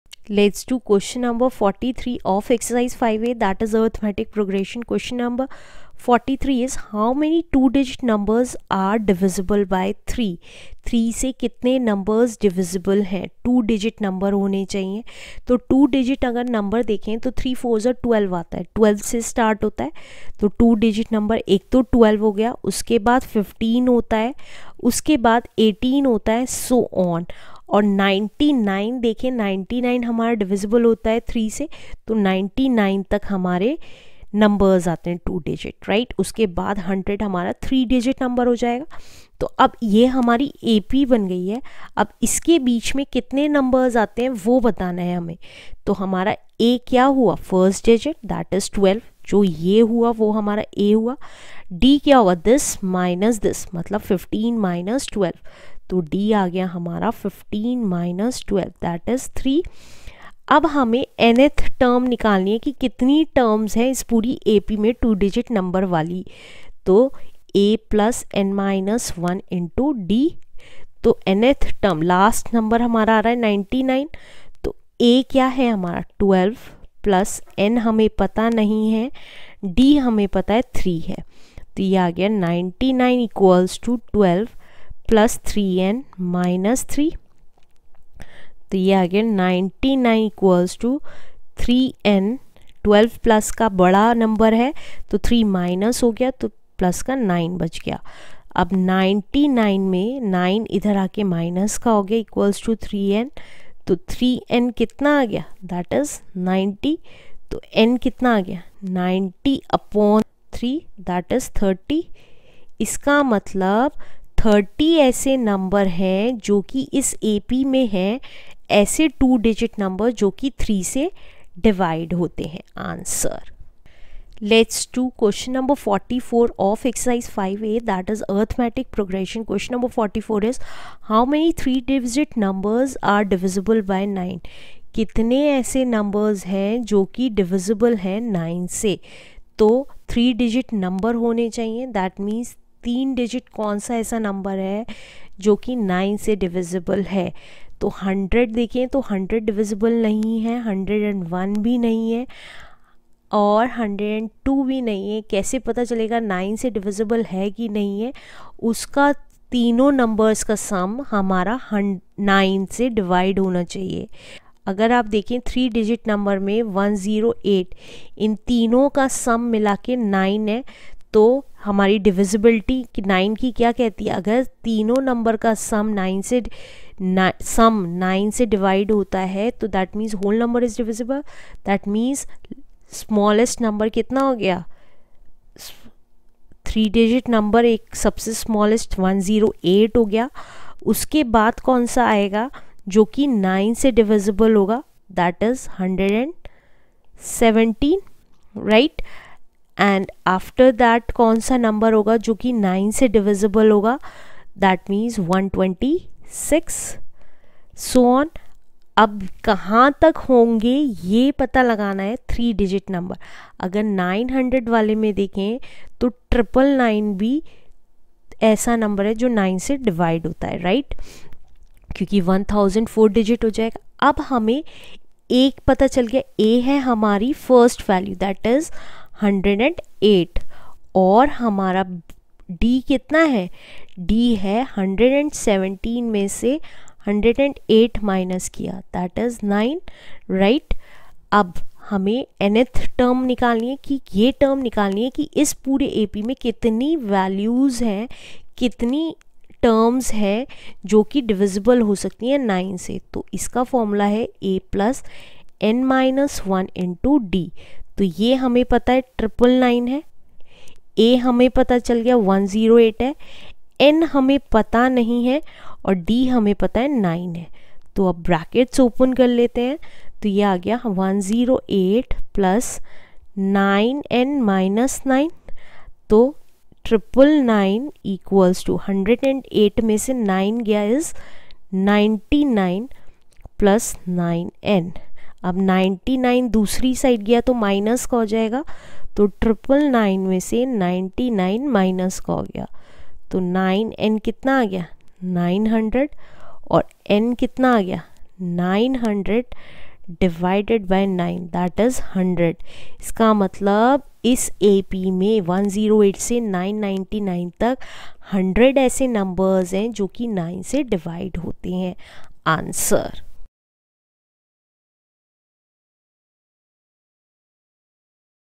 Arithmetic Progression | Class 10 Exercise 5A Question 43 Question 44 | RS Aggarwal | Learn Maths
Book - RS Aggarwal
Class - 10
Chapter - 5 - Arithmetic Progression
Exercise - 5A
Welcome to Learn Maths. This video will solve Question 43 and Question 44 of exercise 5A. Please share your queries and feedback in the comments section.

Regards,
Garima Agarwal